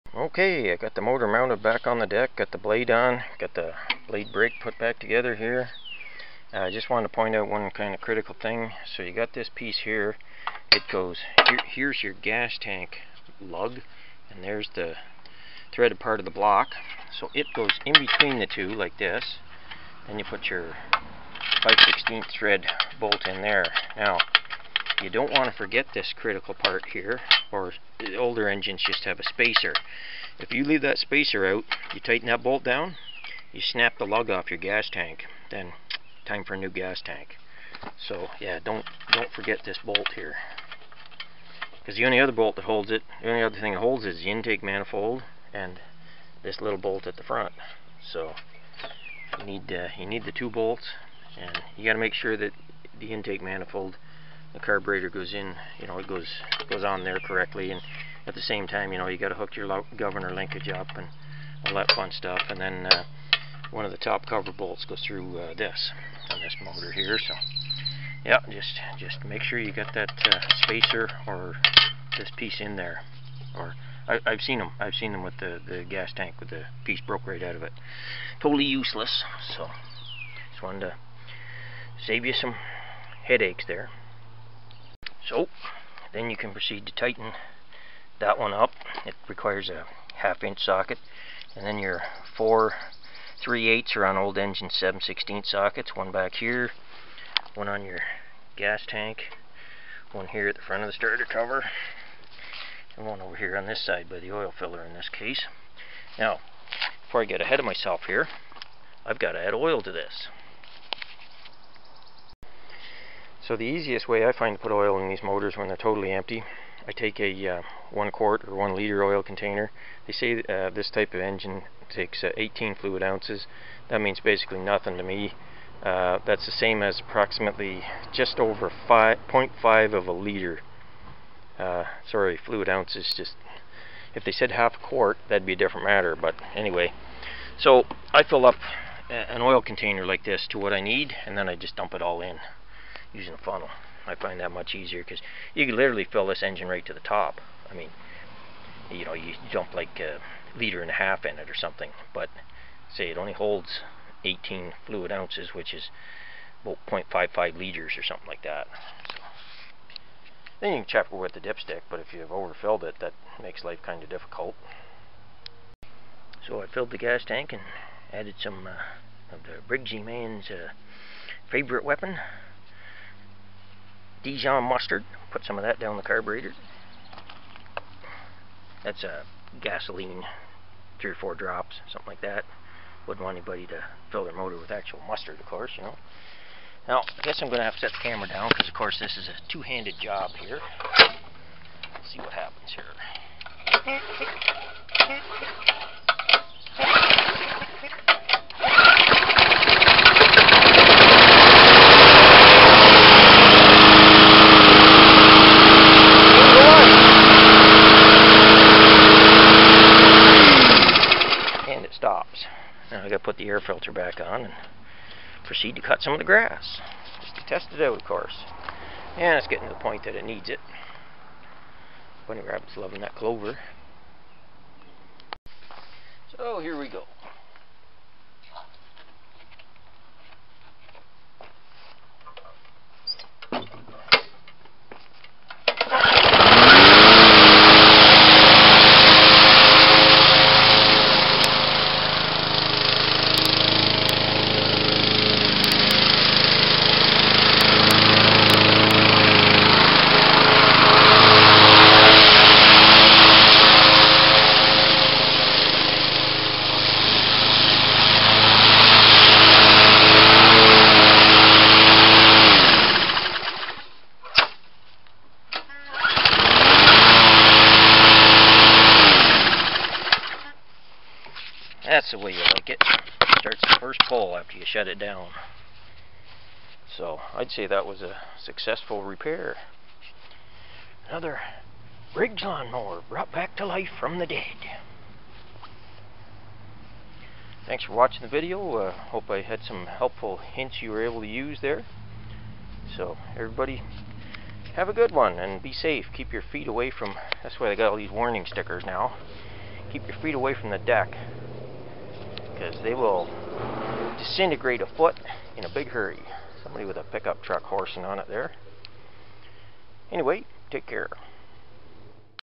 Curb Find Briggs Push Mower Part 3
Final assembly, oil and gas fill up, and away we go cutting. This lawn mower actually works a bit better than my own, so I cut my longest grass with it.